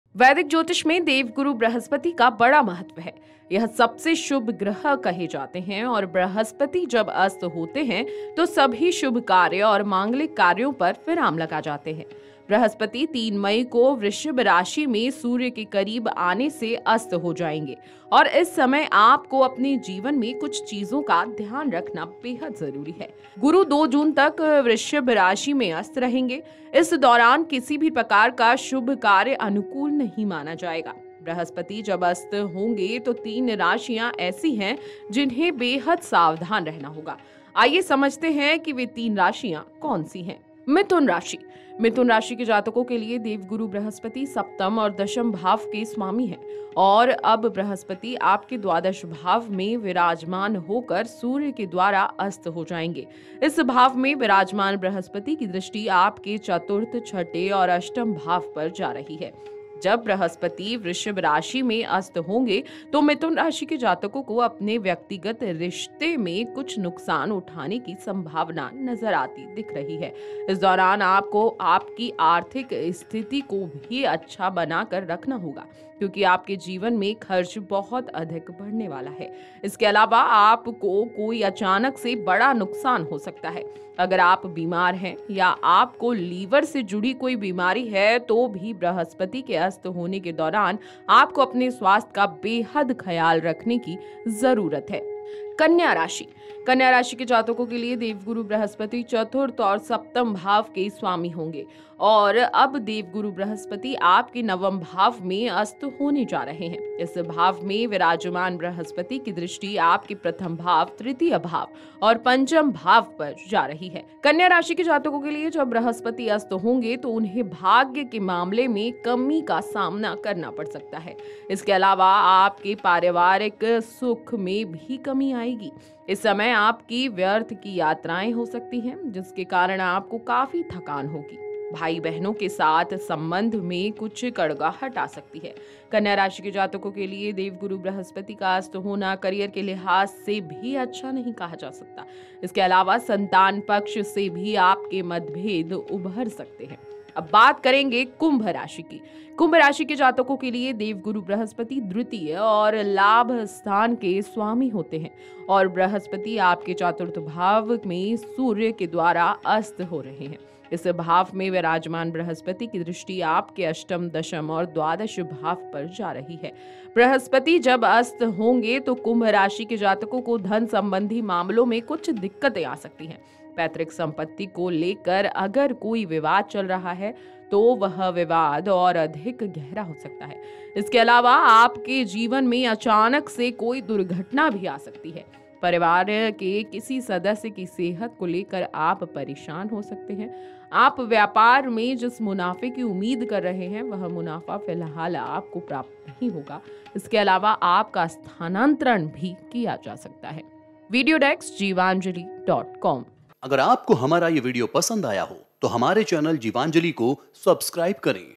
Brhaspati Asta 2024: बृहस्पति के अस्त होने से इन राशियों की बढ़ेगी परेशानी
वैदिक ज्योतिष में देवगुरु बृहस्पति का बड़ा महत्व है। यह सबसे शुभ ग्रह कह गए हैं और बृहस्पति जब अस्त होते हैं तो सभी शुभ कार्य और मांगलिक कार्यों पर विराम लग जाता है। बृहस्पति 3 मई को वृषभ राशि में सूर्य के करीब आने से अस्त हो जाएंगे और इस समय आपको अपने जीवन में कुछ चीजों का ध्यान रखना होगा। गुरु 2 जून तक वृषभ राशि में अस्त रहेंगे। इस दौरान किसी भी प्रकार का शुभ कार्य अनुकूल नहीं माना जाएगा।